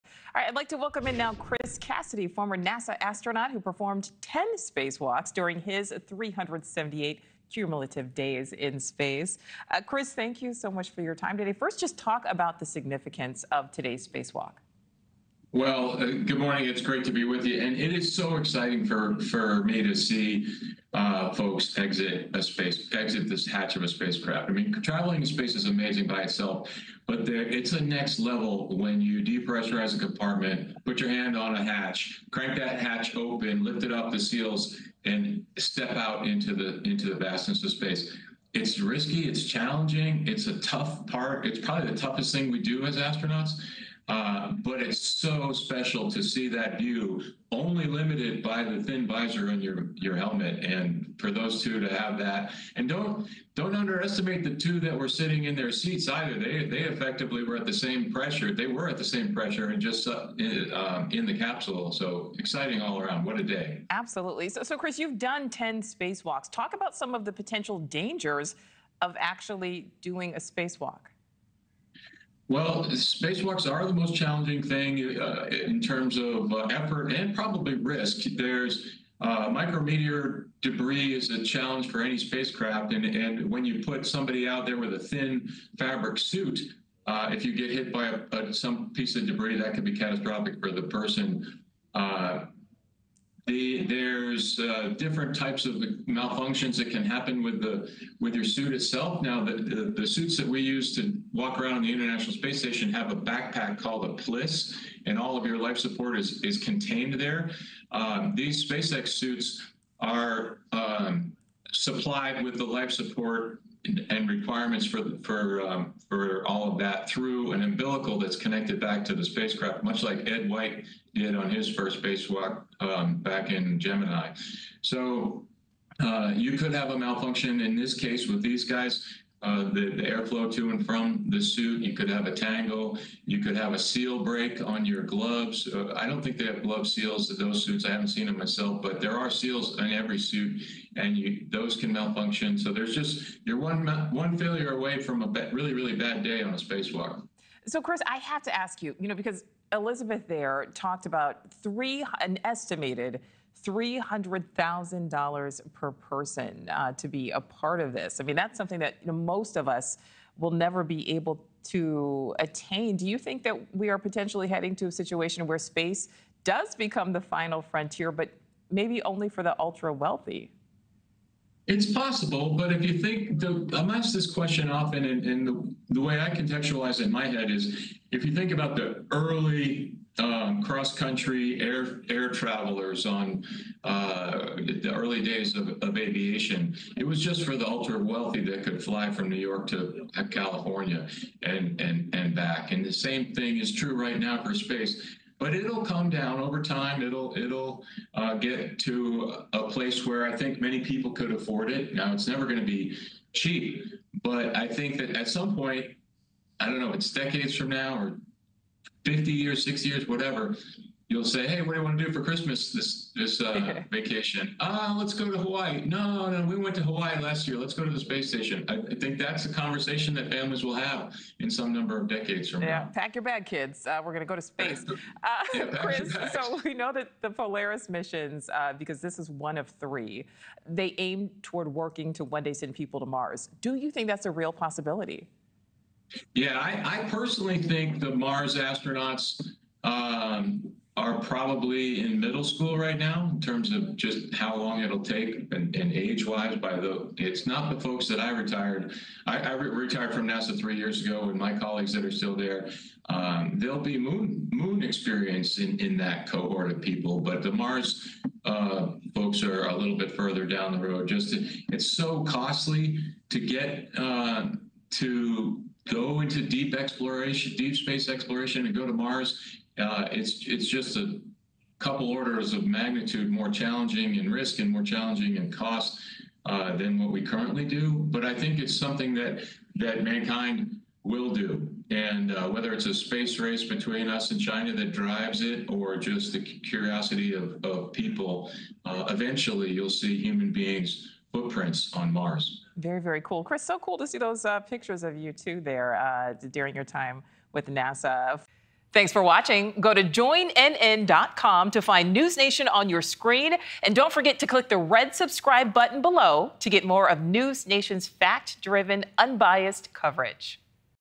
Families will eventually go to space on vacation: Ex-NASA astronaut | NewsNation Now
SpaceX made history by sending tech entrepreneur Jared Isaacman and an all-civilian crew past the International Space Station to conduct a spacewalk. Former NASA astronaut Chris Cassidy, who has gone on 10 spacewalks himself, discusses the danger of conducting a spacewalk. Cassidy predicts it'll take multiple decades, but eventually, families will be visiting space on vacation.